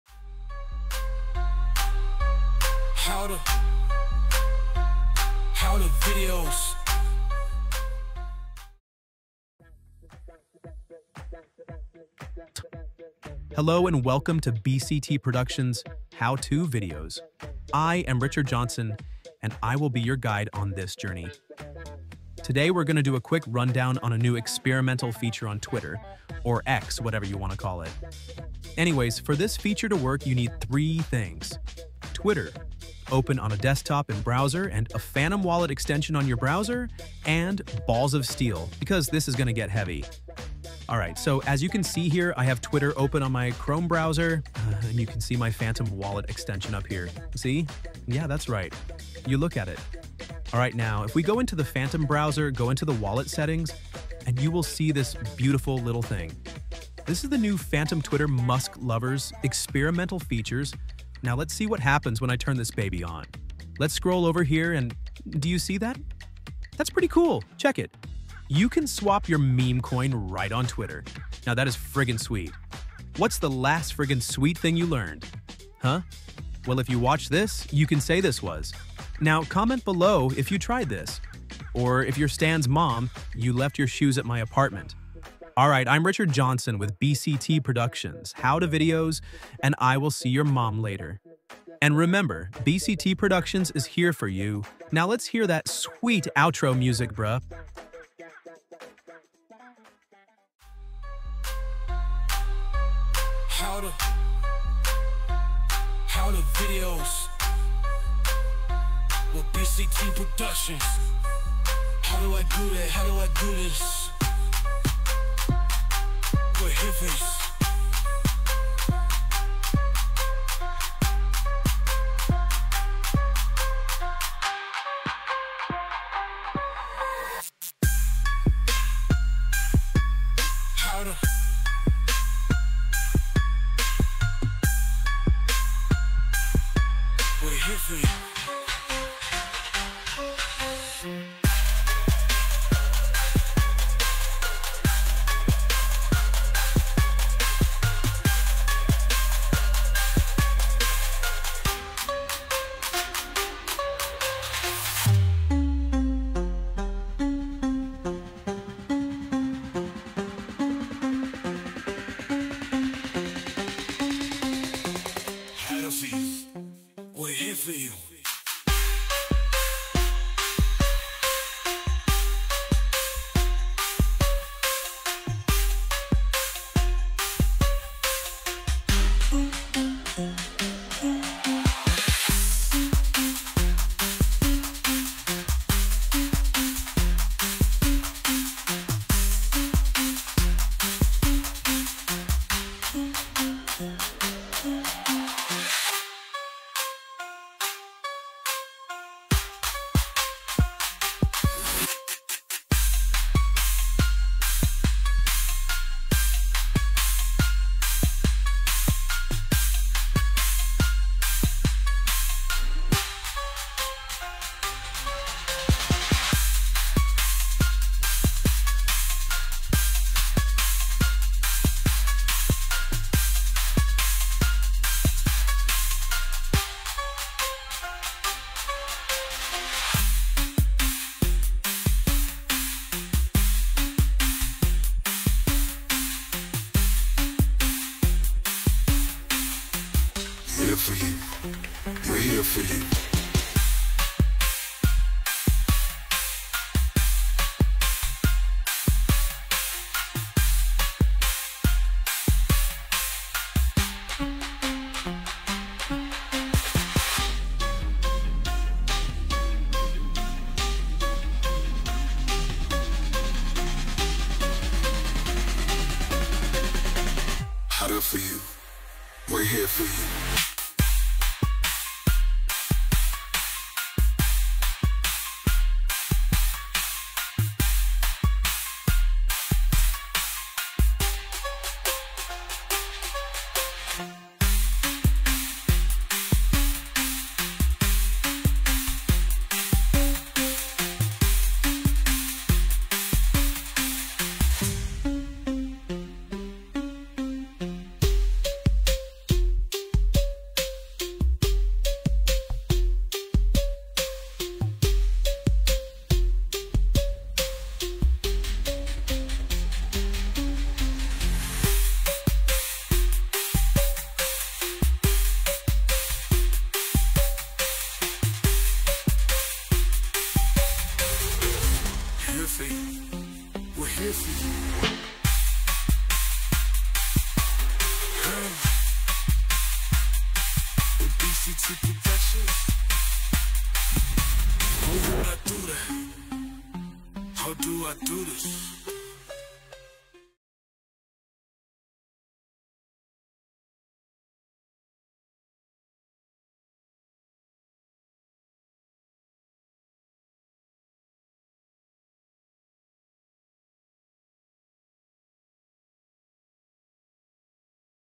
How-To-Video: Phantom Wallet Experimental Features
Discover how to use Phantom’s experimental features to swap memecoins directly on Twitter (now X) in this exclusive how-to video by BCT Productions! Join Richard Johnson as he explores Phantom's integration with Twitter, making crypto trading more accessible than ever. Learn the steps to leverage this game-changing feature for your favorite memecoins. Watch now to stay ahead in the crypto game!